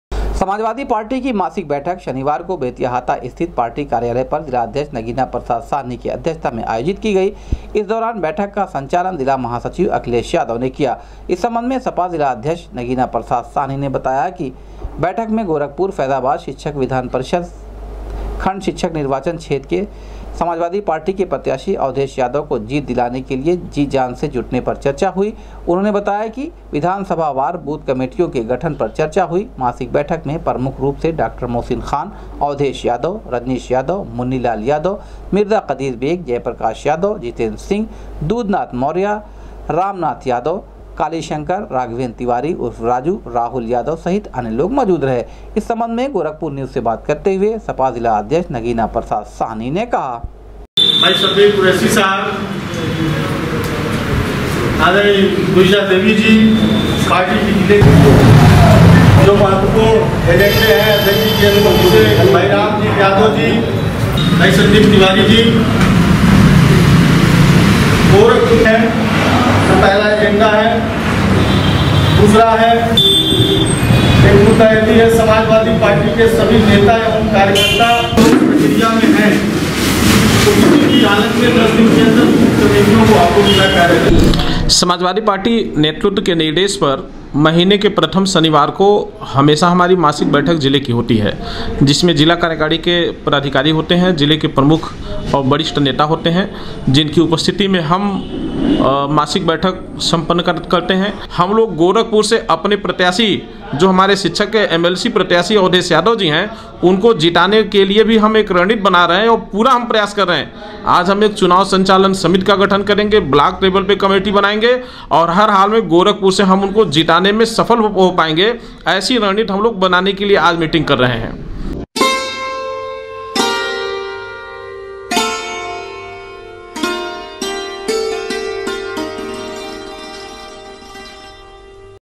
सपा जिलाध्यक्ष की अध्यक्षता में पार्टी कार्यालय पर आयोजित की गई मासिक बैठक, गोरखपुर-फैजाबाद शिक्षक
सपा जिलाध्यक्ष की अध्यक्षता में पार्टी कार्यालय पर आयोजित की गई मासिक बैठक, गोरखपुर-फैजाबाद शिक्षक विधान परिषद खंड शिक्षक निर्वाचन क्षेत्र से समाजवादी पार्टी के प्रत्याशी को जीत दिलाने के लिए बनाई रणनीति